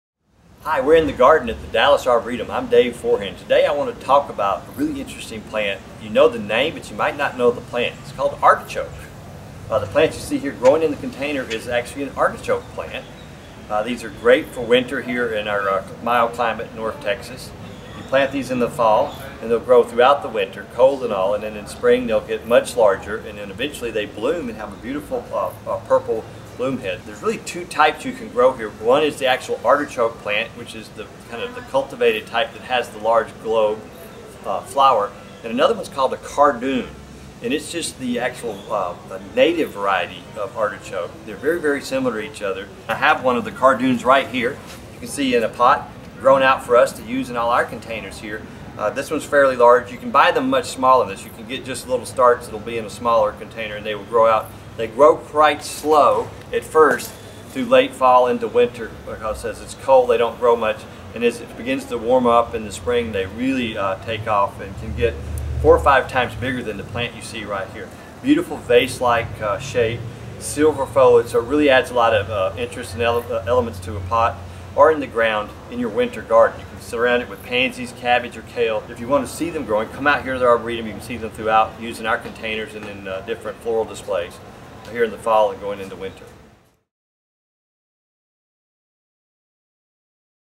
In the Garden with Dave Forehand: Artichokes
Dave Forehand, vice president of gardens, talks about artichokes and cardoons, which are great winter plantings and can be found throughout the garden.